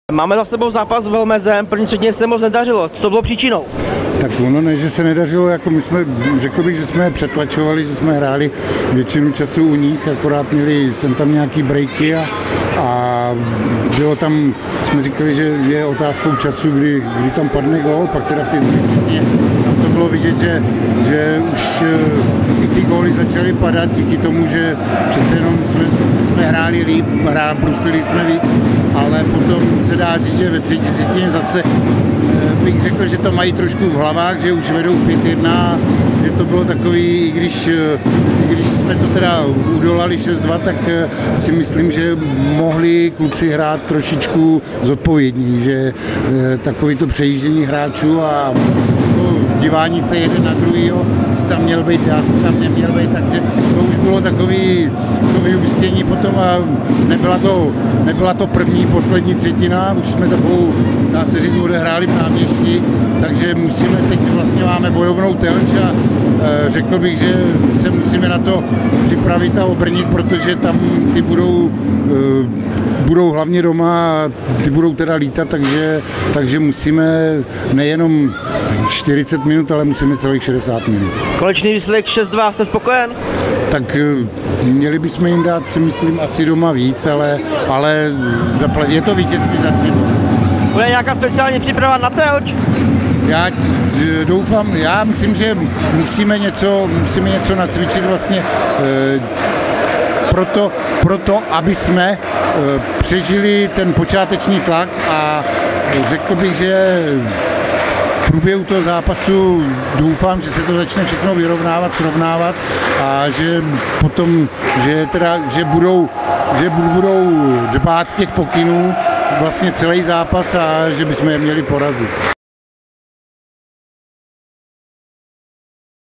HC Pelhřimov: Po zápasový rozhovor s trenérem Oldřichem Válkem
Domácí nasázeli soupeři krásných 6 branek a zvítězil 6:2 nad Velmezem!
Jak zhodnotil zápas trenér HC Pelhřimov Oldřich Válek?